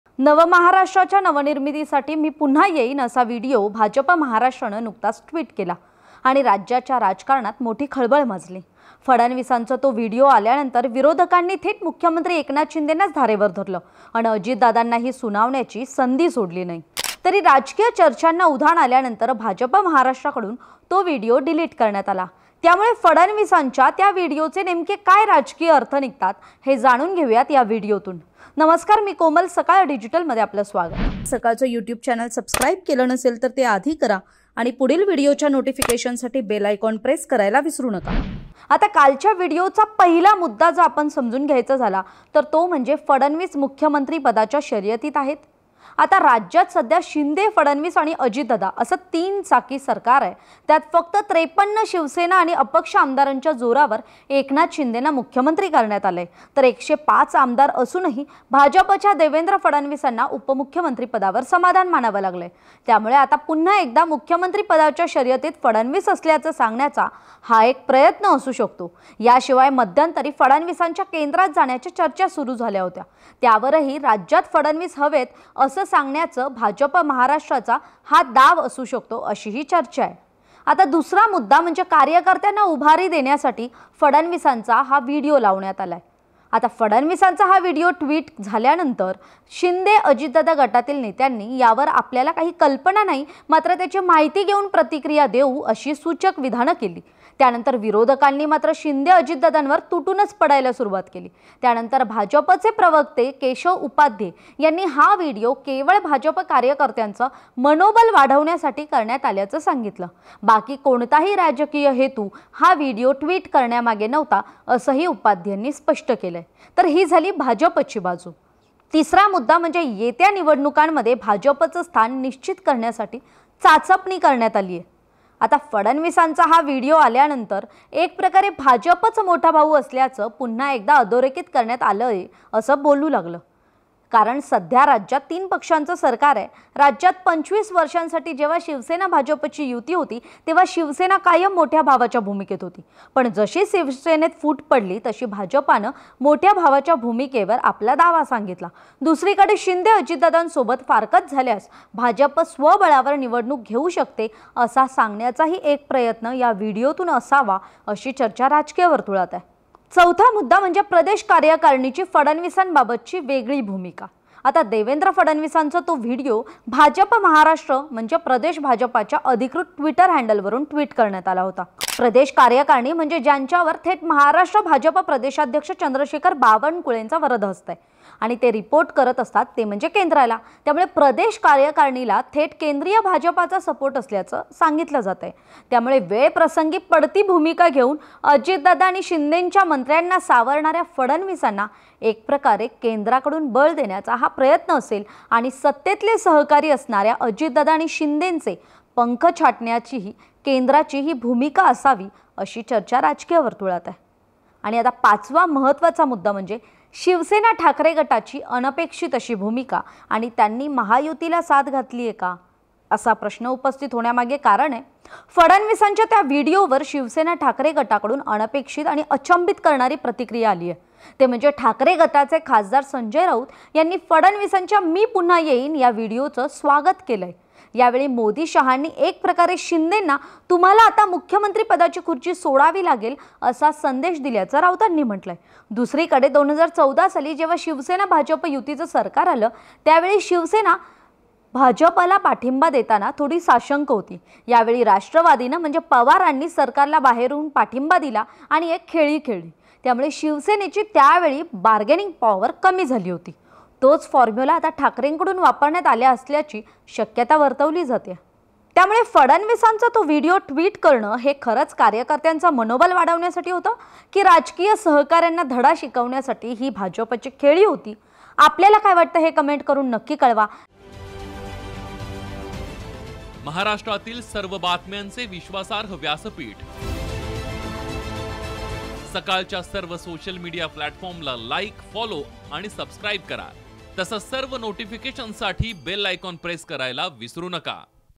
Devendra Fadnavis व्हिडिओवर Uddhav Thackeray यांची खेळी, महायुतीला साद! जाणून घ्या ५ राजकीय अर्थ
नवमहाराष्ट्राच्या नवनिर्मितीसाठी मी पुन्हा येईन, असा व्हिडिओ काल भाजप महाराष्ट्रानं ट्विट केला आणि राज्याच्या राजकारणात मोठी खळबळ माजली. फडणवीसांचा तो व्हिडिओ आल्यानंतर विरोधकांनी तर थेट मुख्यमंत्री एकनाथ शिंदेंनाच धारेवर धरलं. तर, अजितदादांनाही सुनावण्याची संधी सोडली नाही. तरी, राजकीय चर्चांना उधाण आल्यानंतर भाजप महाराष्ट्राकडून तो व्हिडिओ डिलीट करण्यात आलाय. त्यामुळे फडणवीसांचा त्या व्हिडिओचे नेमके काय राजकीय अर्थ निघतात, हे जाणून घेऊयात या व्हिडिओतून